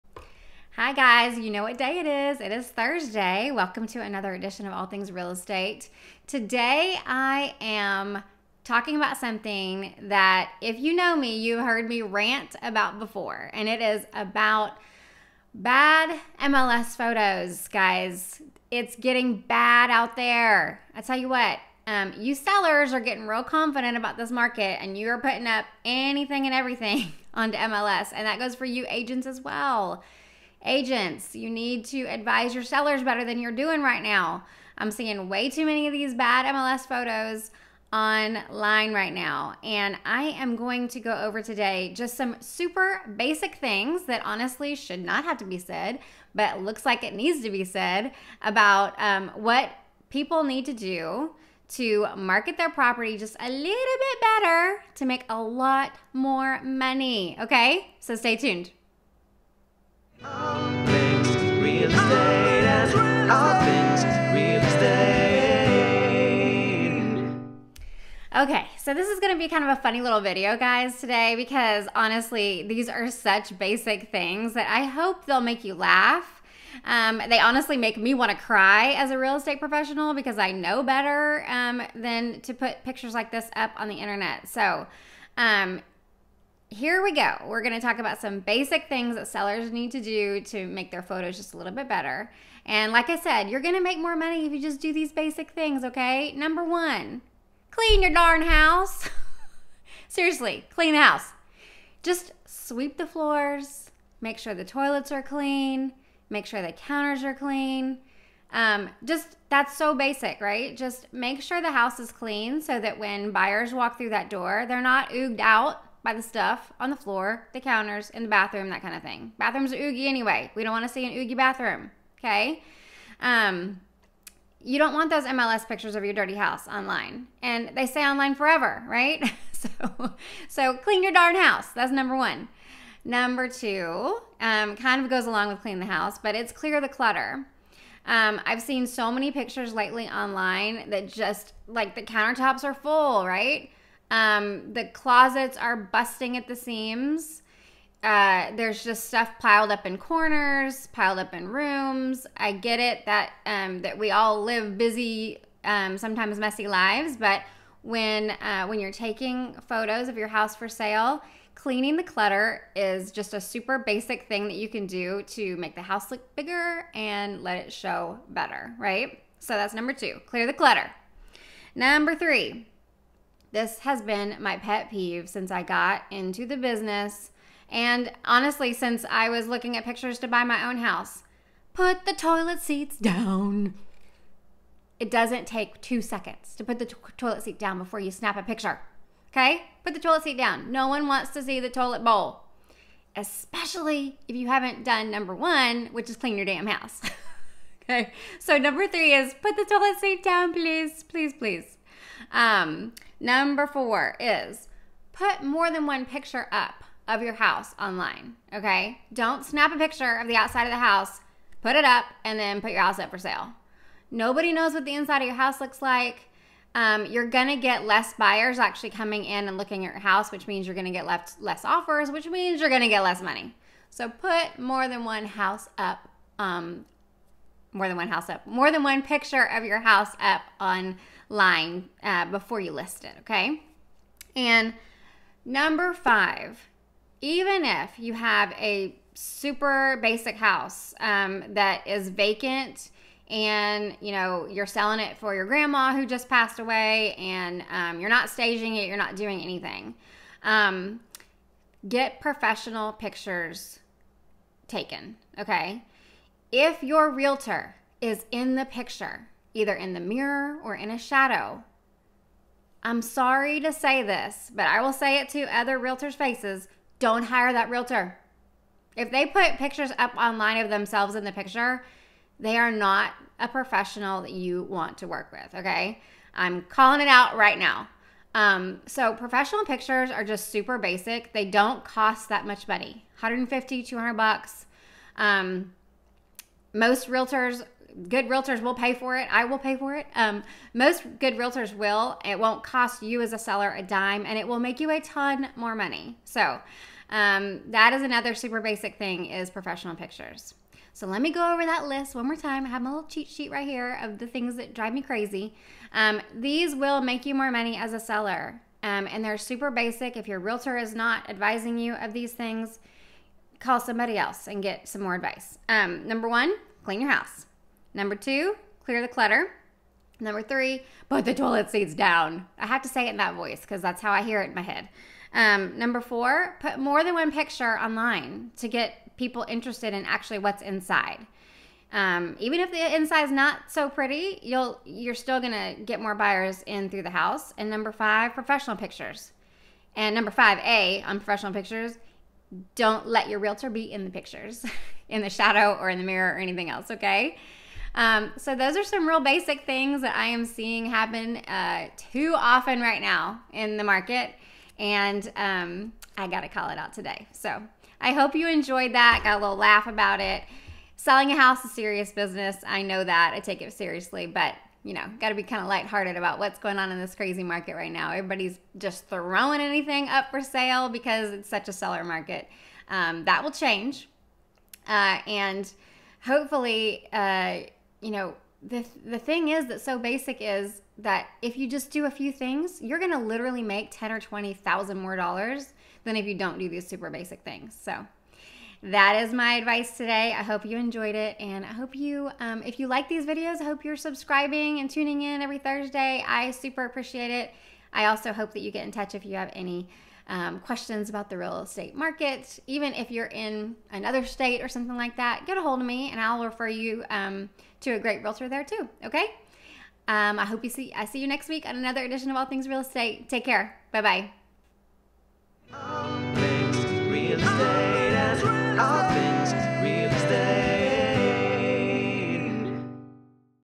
5 Easy Ways For Sellers To Make More Money EVEN In This Market || Heather Sims Realtor
5 Easy Ways For Sellers To Make More Money EVEN In This Market

I've noticed A LOT of listings lately that are NOT ready to go on market and it honestly makes me mad (I've included some bad examples too!), because I feel like these sellers simply aren't getting good advice. Even if you have a house that isn't the prettiest, isn't updated, isn't going to be HGTV-worthy no matter what a seller does, there are still BASIC things that can and should be done because they WILL make a seller more money. So check this out for those simple 5 things, PLUS check out those examples of bad MLS photos where those things have not been done!